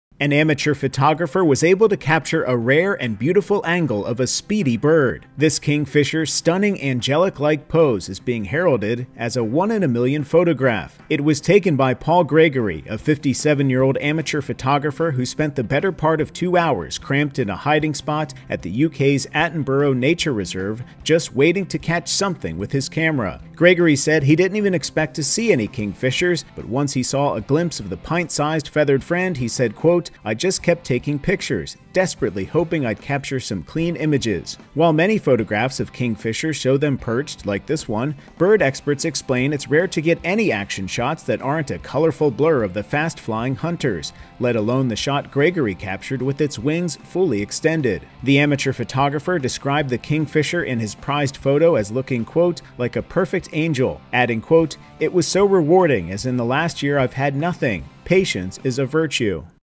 ‘One-in-a-Million’ Picture of Kingfisher Bird Snapped by Amateur Photographer
An amateur photographer was able to capture a rare and beautiful angle of a speedy bird. Buzz60’s Justin Kircher has the story.